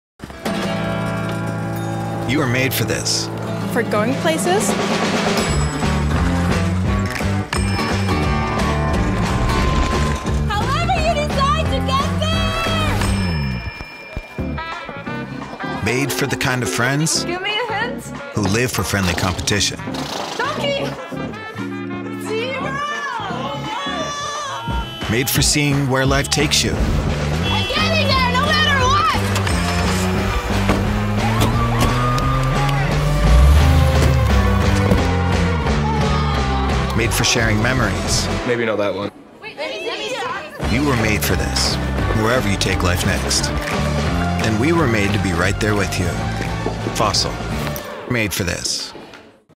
Fossil - Made For This
Agency  : Mekanism
Campaign : Made For This
Advertiser : Fossil
Brand  : Fossil
Length  : 49 seconds
Posted   : Sept 1, 2023

In this Movie Ad we’ll walk you through:
Celebrates the everyday moments that make life worth living.

TIMESTAMPS :
00:00 Fossil - Made For This
Plot :
It shows people of all ages and backgrounds enjoying their lives, from traveling the world to spending time with loved ones. The ad highlights how Fossil watches and accessories can help people capture and remember these moments.